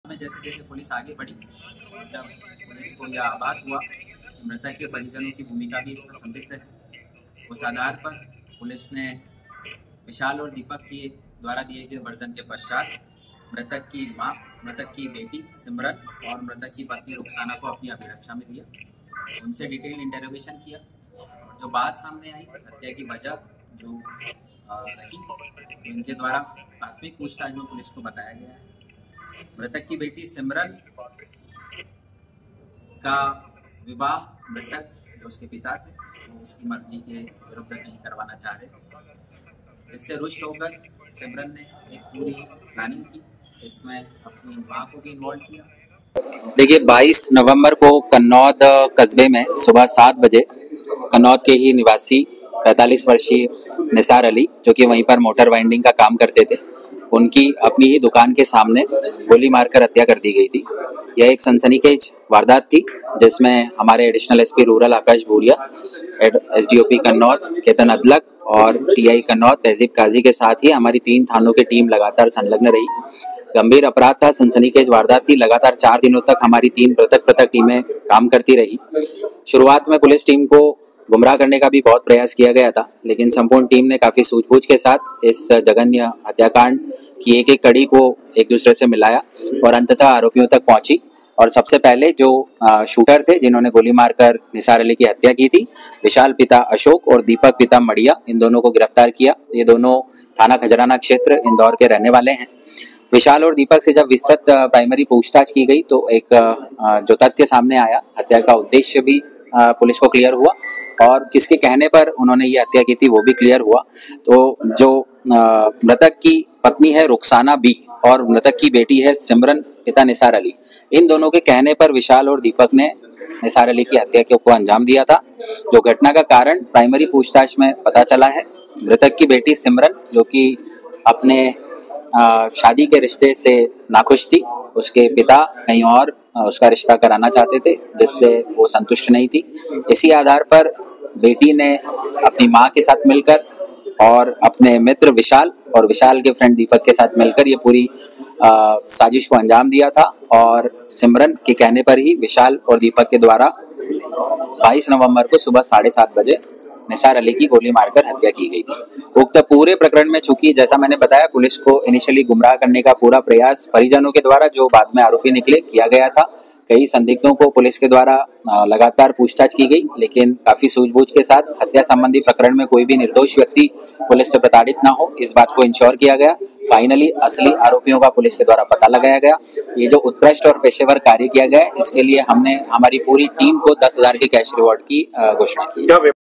नाराज पुत्री ने मां के साथ मिलकर रची थी पिता के हत्या की साजिश। देवास पुलिस ने किया खुलासा।
दूसरी जगह शादी करवाने की बात से नाराज पुत्री ने मां के साथ मिलकर की रची थी पिता के हत्या की साजिश। ब्लाइंड मर्डर में मृतक की पुत्री व पत्नी ही निकली मास्टर माइंड। देवास पुलिस ने किया कन्नौद गोलीकाण्ड का 04 दिन में पर्दाफाश।